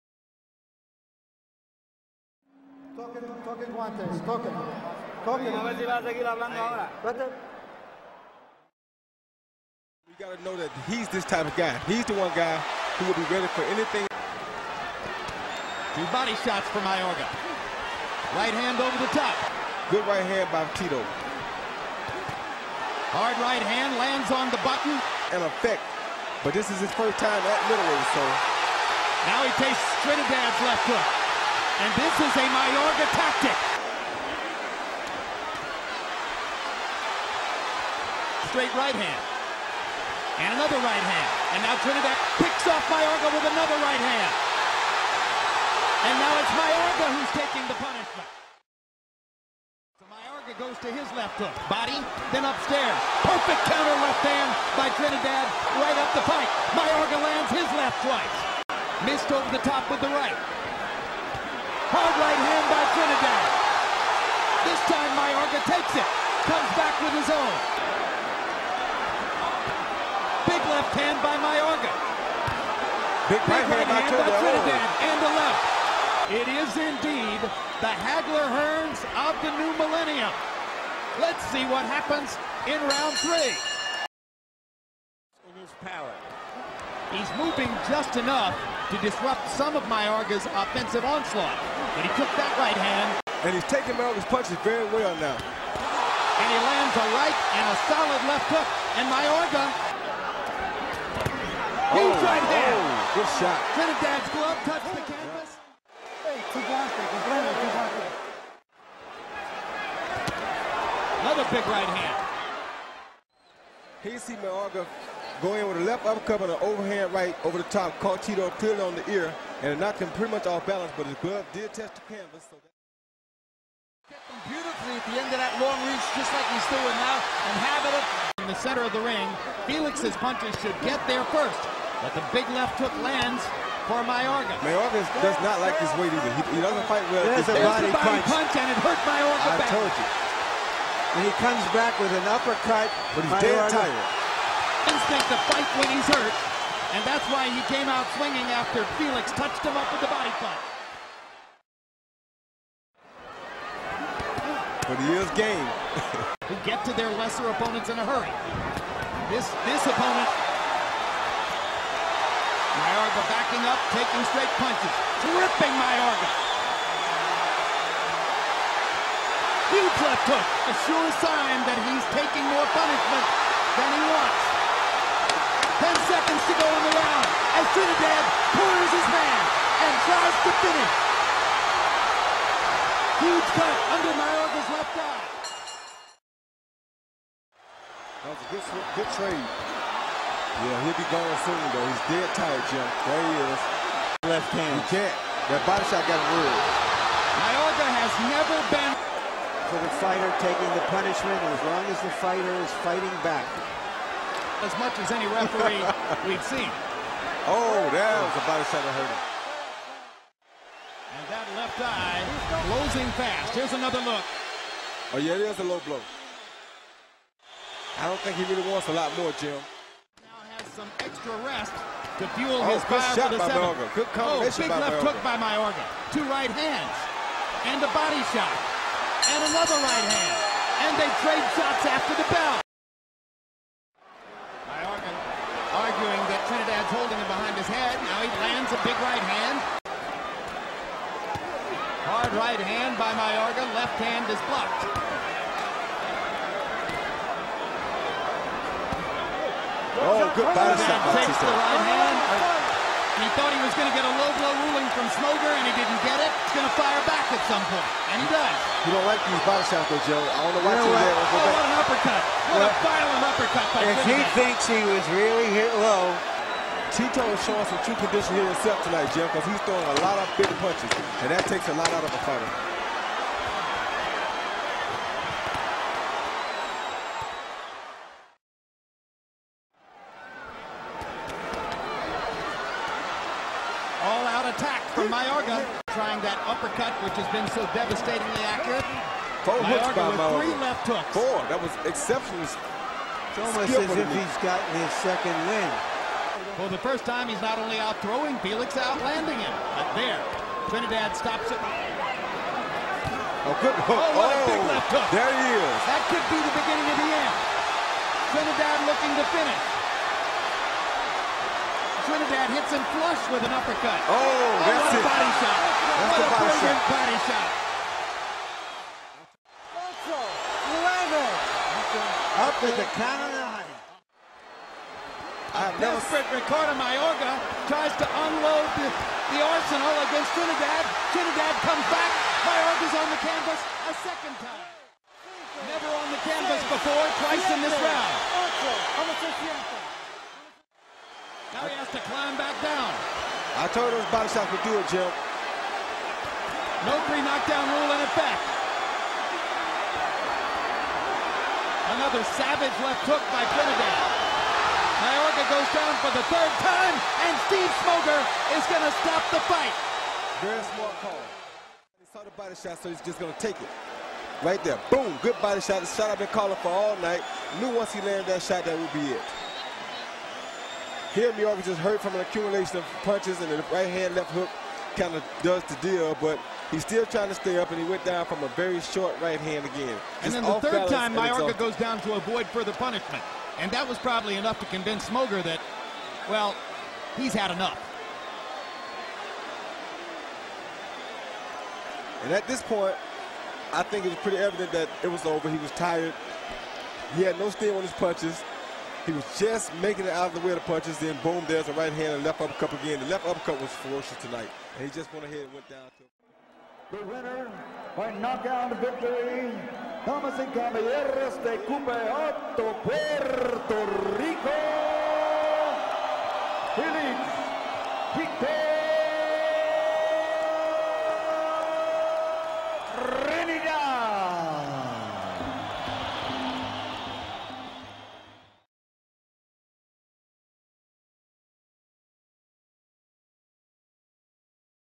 Felix Trinidad vs Ricardo Mayorga - Highlights (Amazing Fight & KNOCKOUT)
Highlights of the highly exciting fight between Tito Trinidad and Ricardo Mayorga in one of Tito's most memorable wins!